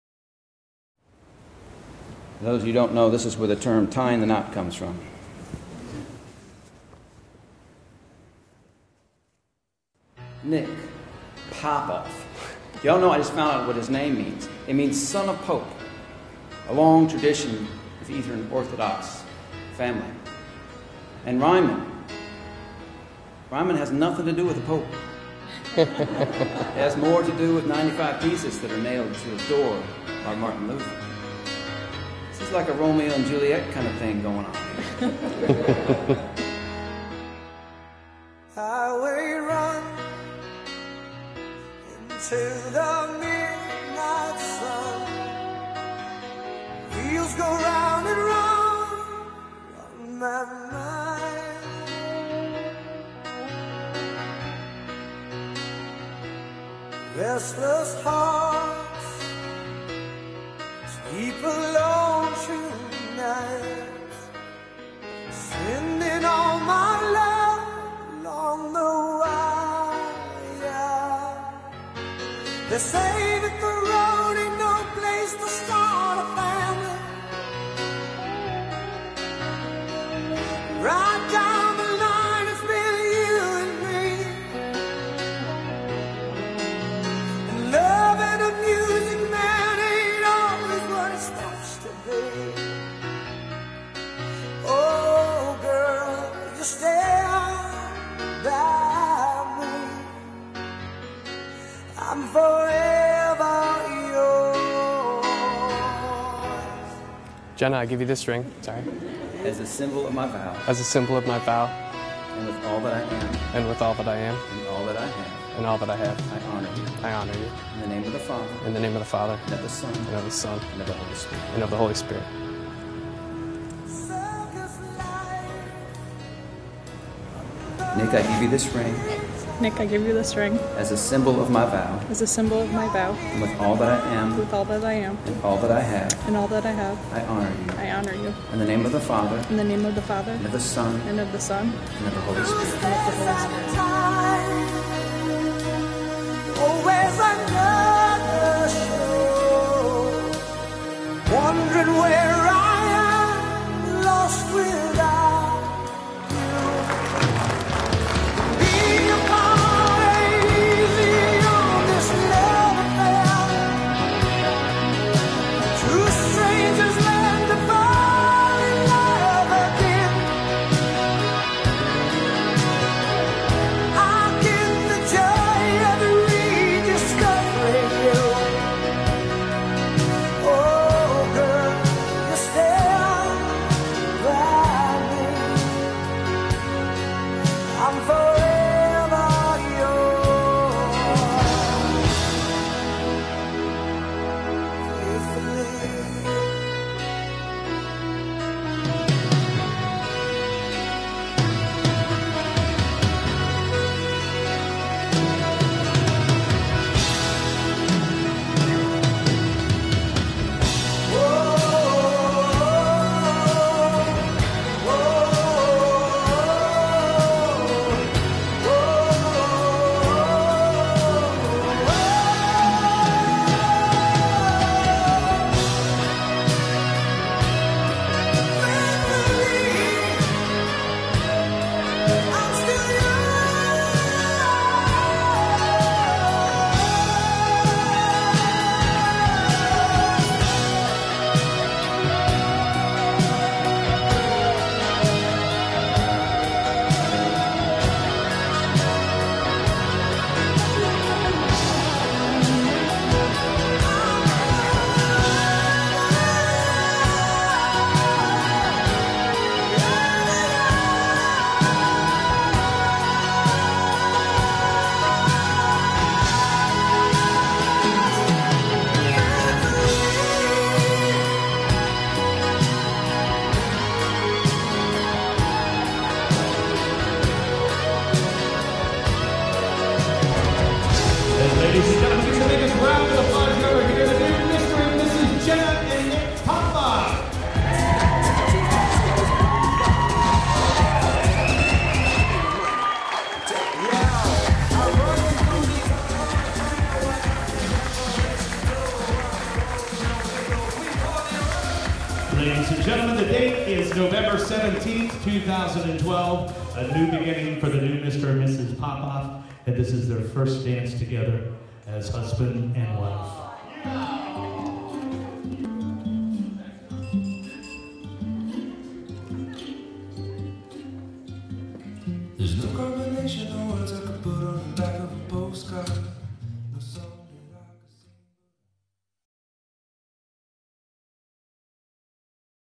Jenna & Nicholas - Wedding Highlight Film - St. Marks Episcopal Church, Marco Marriott
Jenna & Nicholas - Wedding Highlight Film - St. Marks Episcopal Church and Marco Mariott, Marco Island Florida,